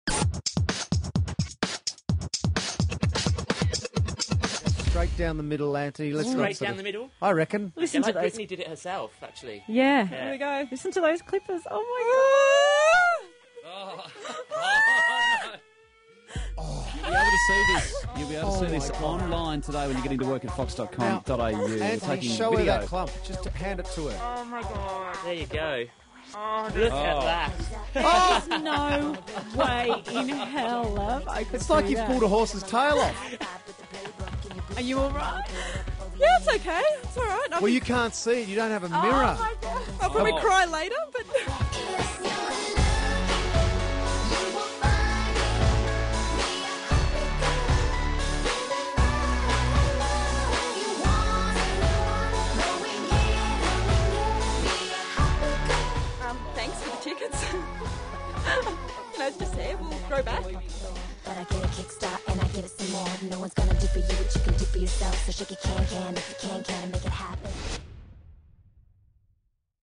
Head Shave For Britney!
Meet the girl who shaved her head to win Britney Love Seat tickets! How will her mum react?